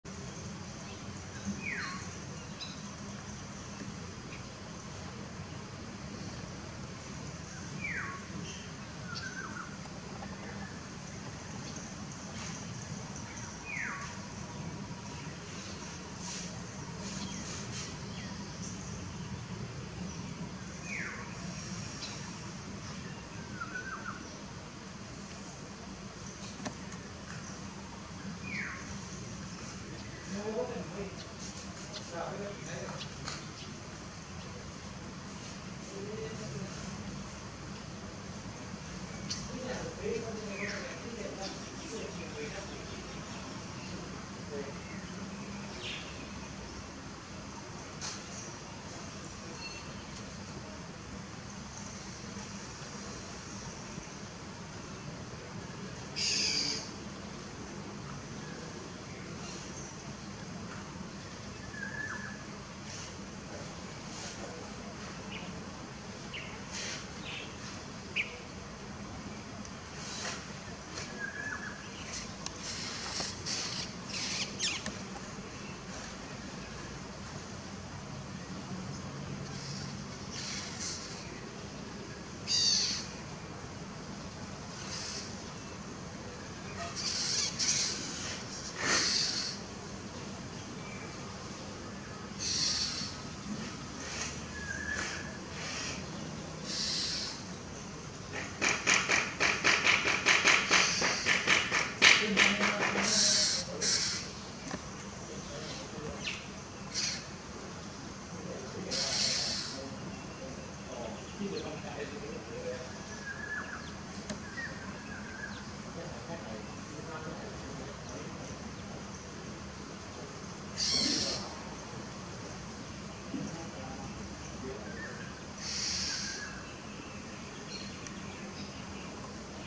Crested Honey Buzzard couple drying after the rain
- 5/10/23
- 6ish pm after their bath.